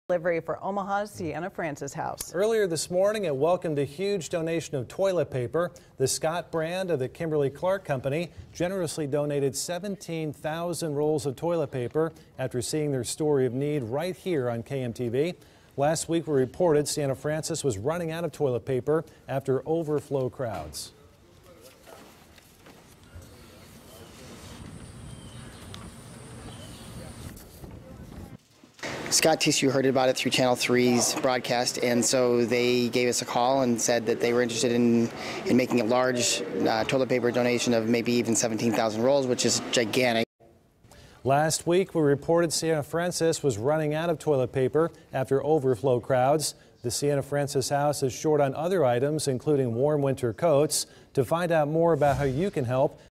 Toilet paper company donates 17,000 rolls to Sienna/Francis House
The shelter welcomed a huge donation of toilet paper from the Scott brand of the Kimberly-Clark Company, which donated 17,000 rolls of toilet paper.